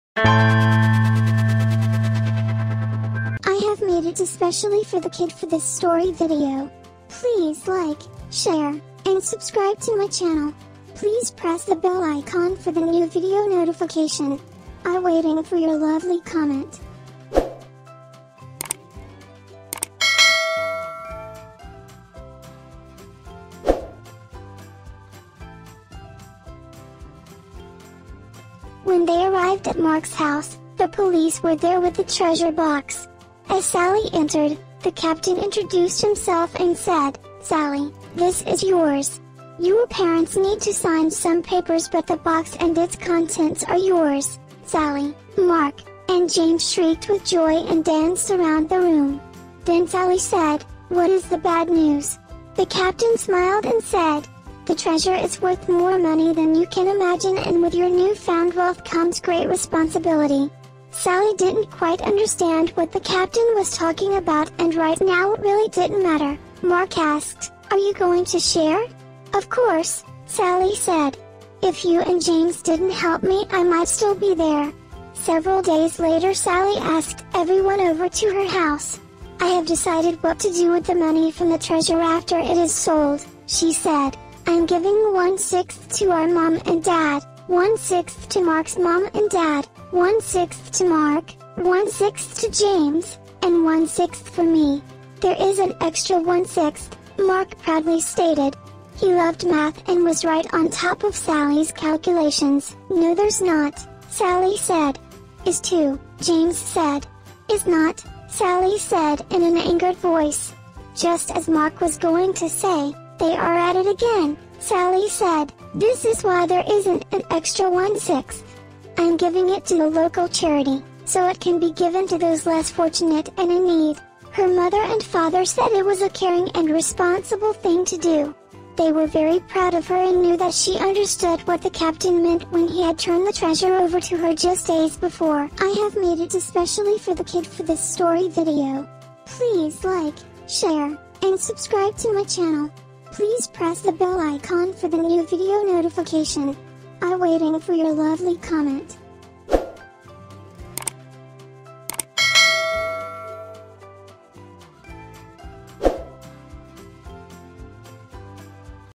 A simple game of hide-and-seek - exciting kid story 09
A simple game of hide-and-seek leads the children into the adventure of their lives.  An exciting story that will have little readers turning the pages, eager to know what comes next.